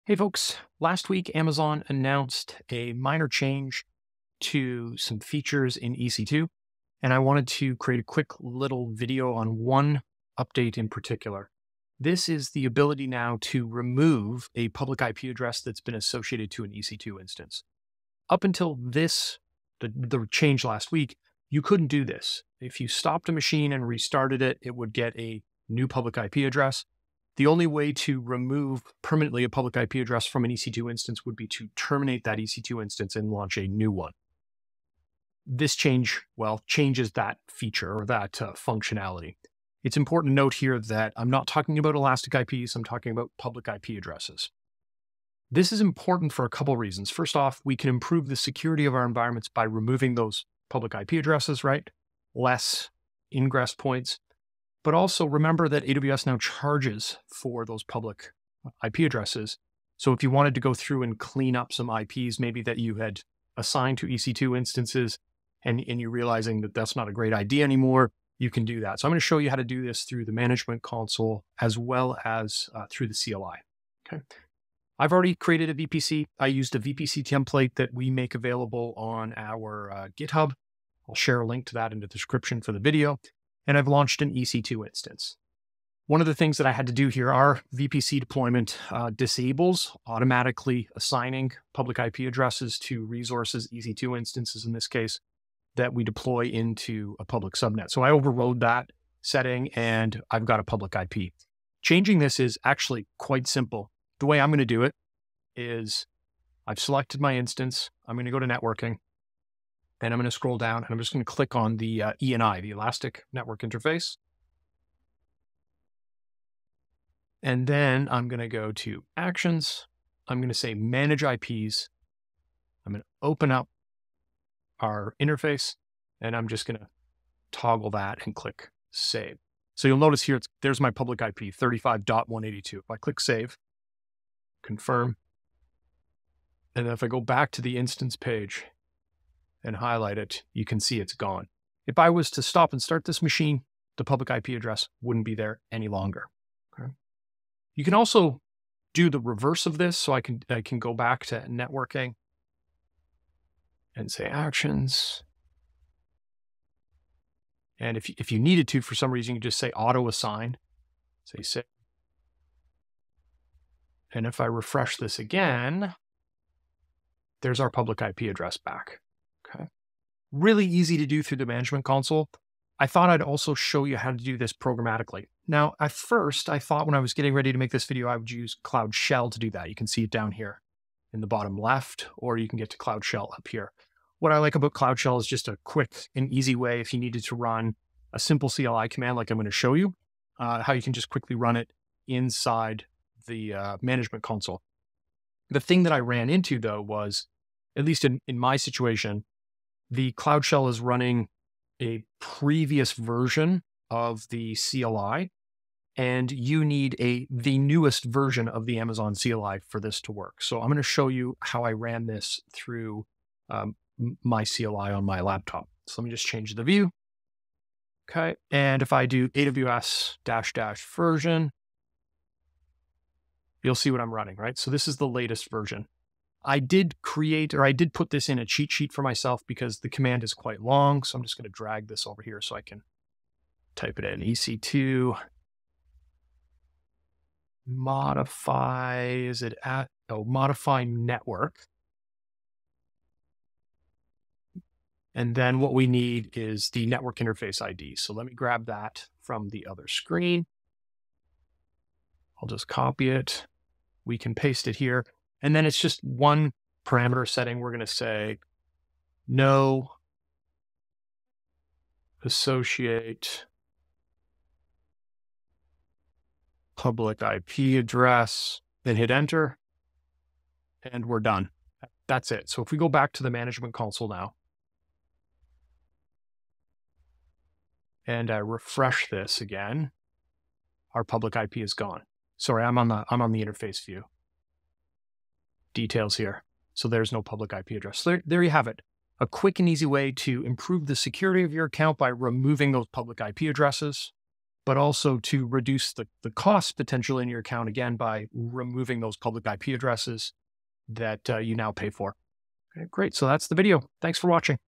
How to Remove Public IP Addresses from AWS EC2 Instances
In this video, learn how to remove public IP addresses from EC2 instances with just a few clicks. This update is important for two reasons - improving security by reducing ingress points and lowering costs since AWS now charges for public IPs.

You'll see a step-by-step demo of removing a public IP address from an existing EC2 instance using the AWS Management Console and the AWS Command Line Interface (CLI). The video covers disassociating the public IP by modifying the Elastic Network Interface settings in the console.

It also shows the AWS CLI command for programmatically removing the public IP from an instance. This new EC2 feature provides an easy way to clean up unused public IPs and tighten security on your EC2 resources. Whether using the console or CLI, this quick tutorial gets you hands-on practice with this newly added capability.